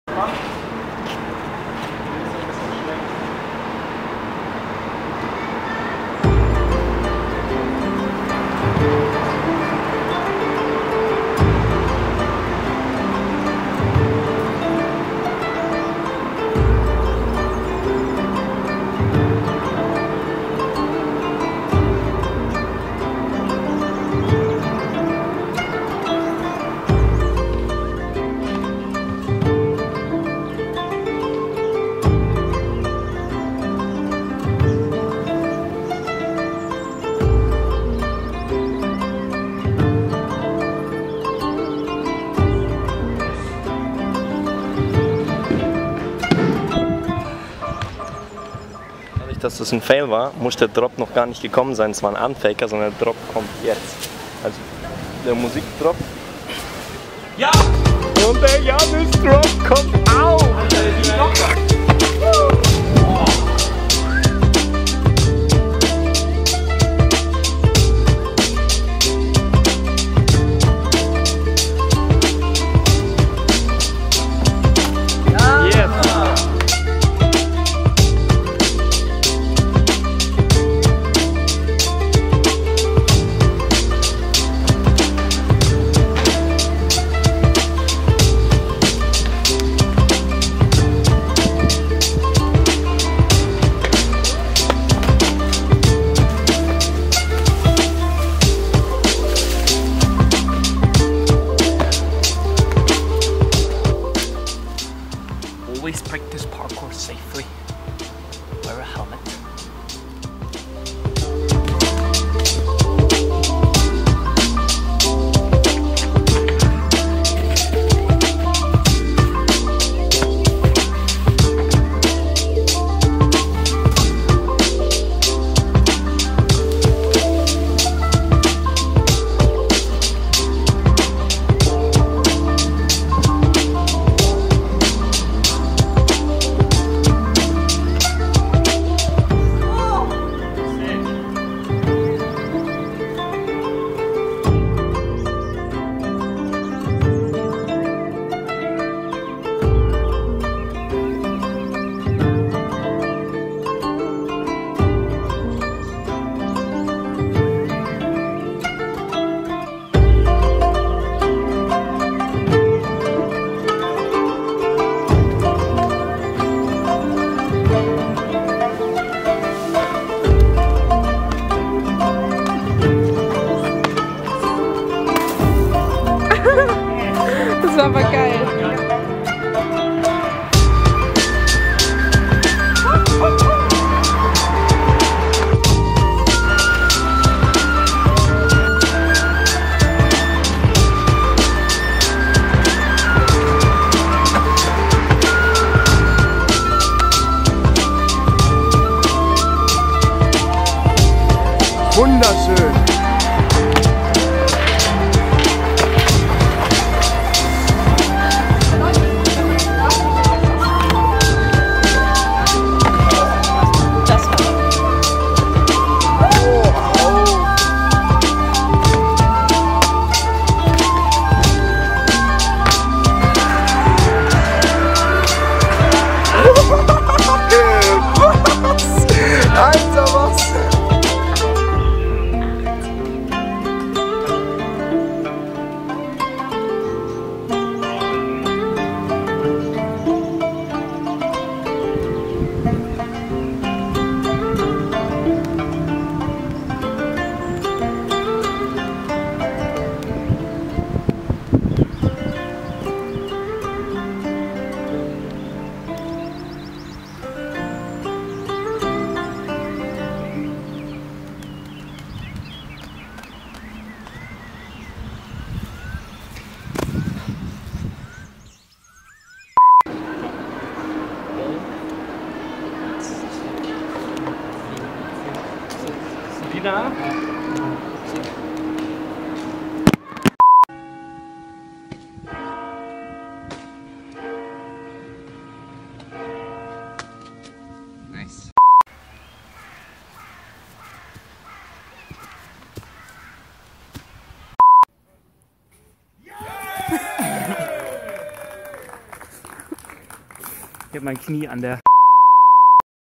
Jannis Schauer 2017
Alternative titel: Masterscoot cork compilation

Thanks for filming: tommenik, mad, luaaadwig, amos, schrame, lensch, holgi, erk, kraut, ando ament, dennis, leroy